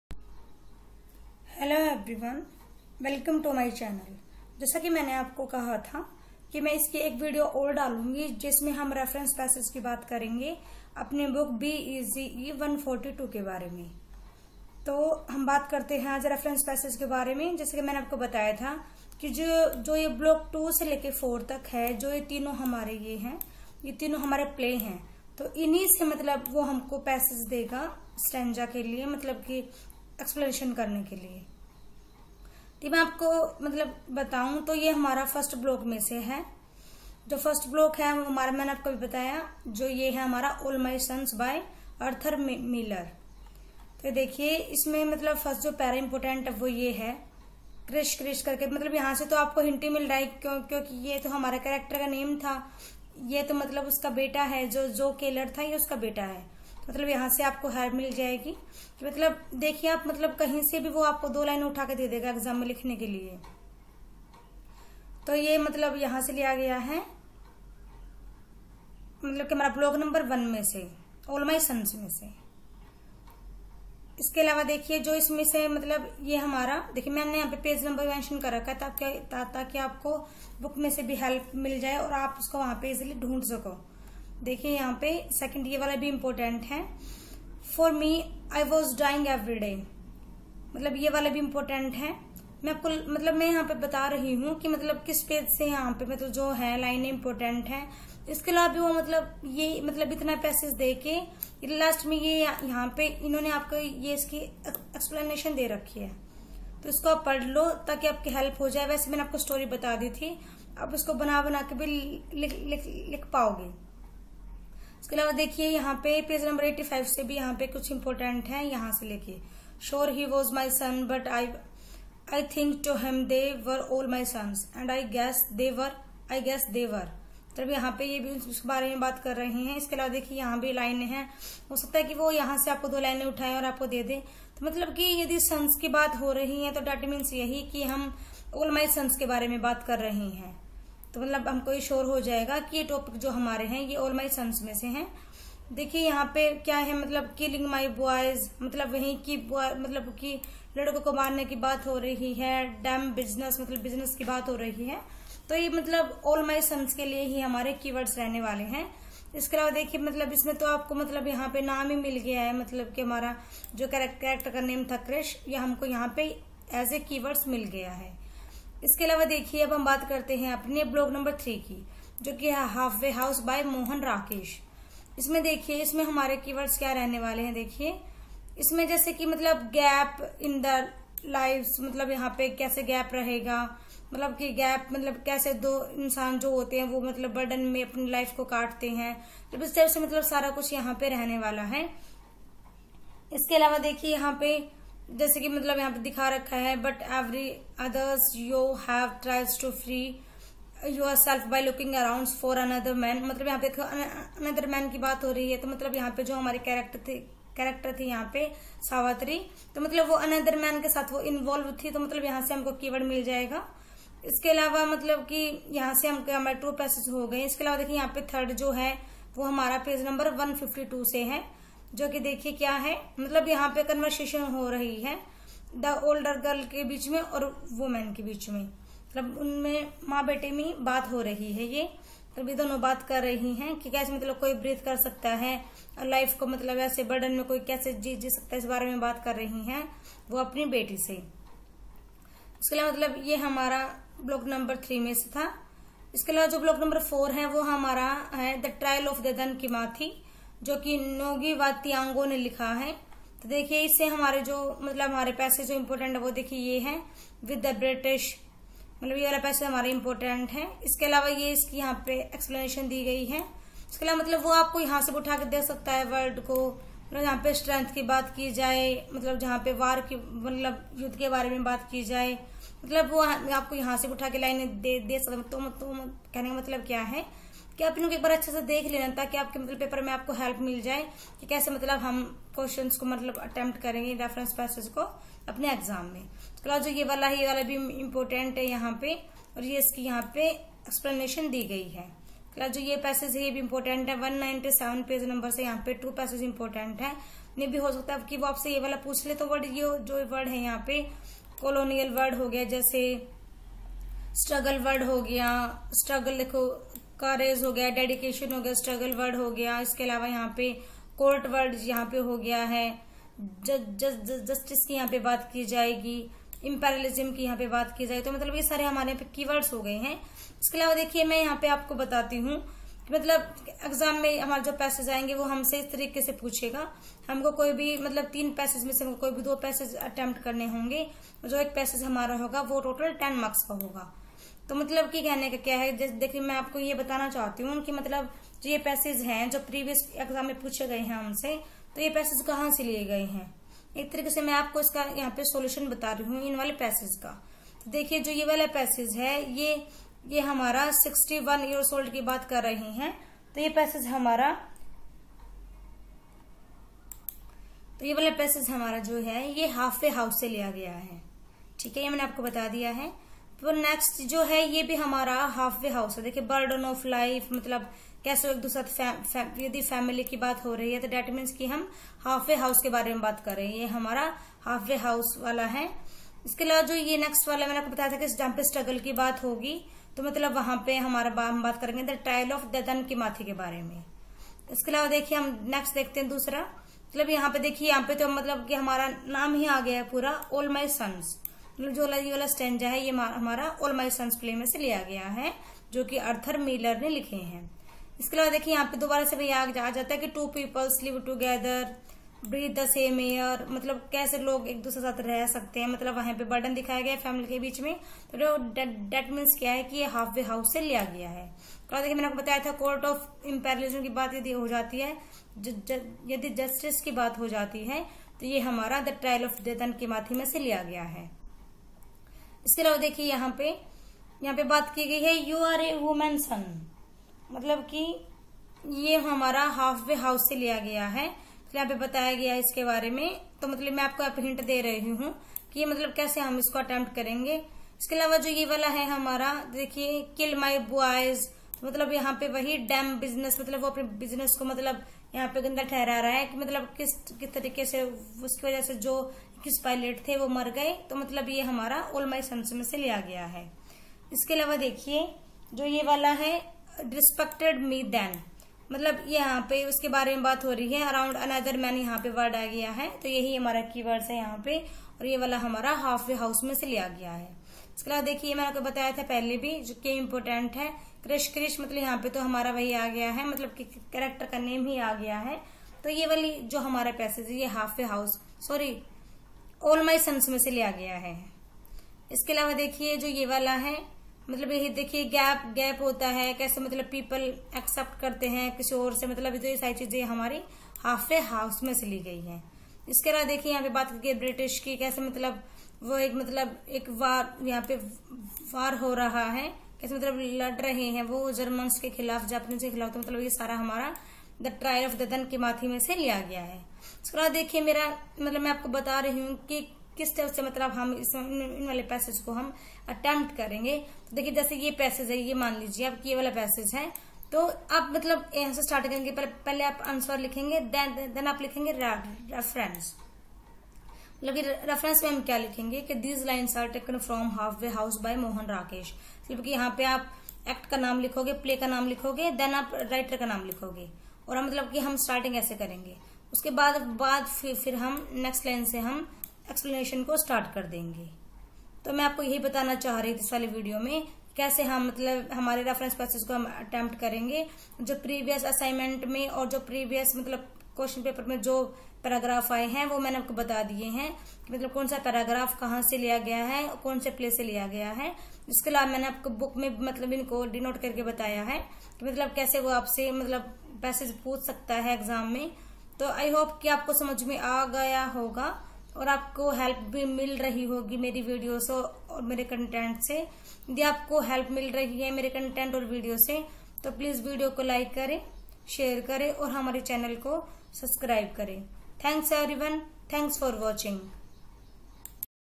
BEGE- 142 (Understanding drama)I Learn how to attempt reference passage in exam I#ignoubagwithnisha
BEGE- 142 (Understanding drama)I Learn how to attempt reference passage in exam I Important reference passage for exam I